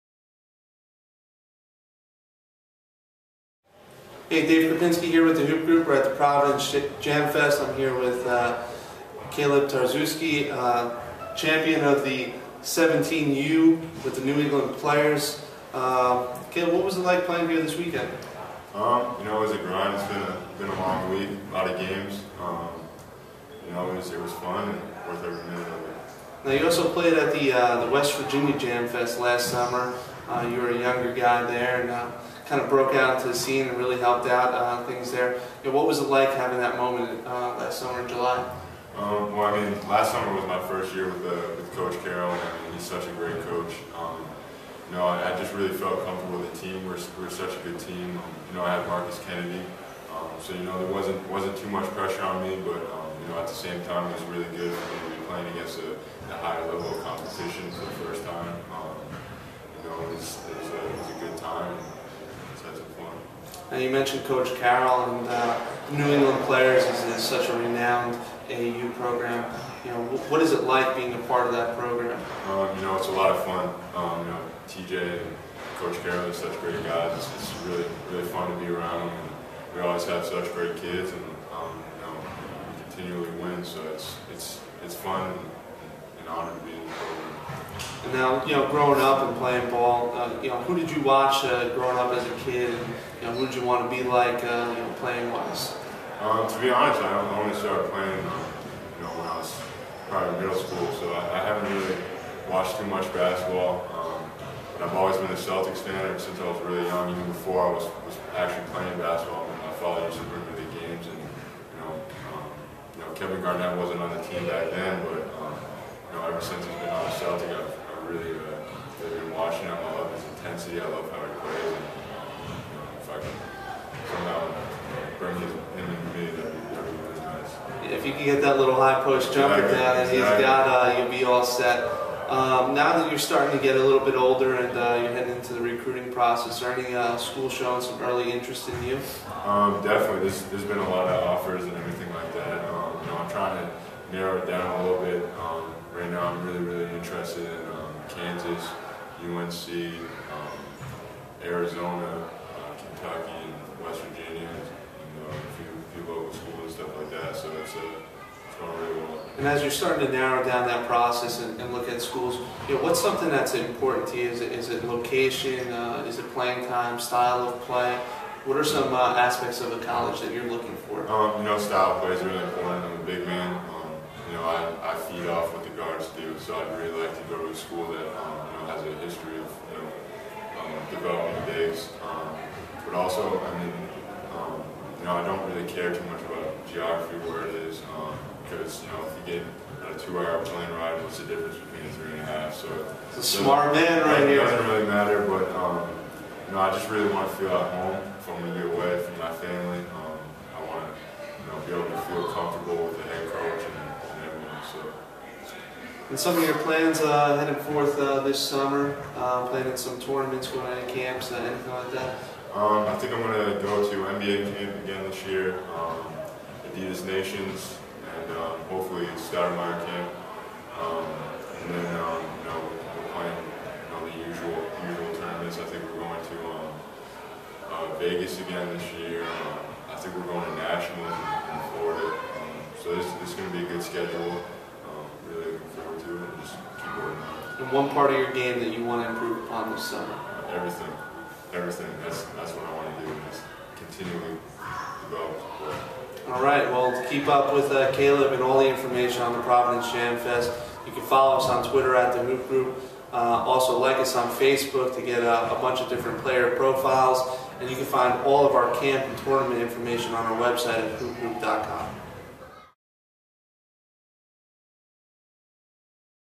Kaleb Tarczewski at Hoop Group Providence Jam Fest
Arizona Center Kaleb Tarczewski sits down at the Hoop Group Pittsburgh Jam Fest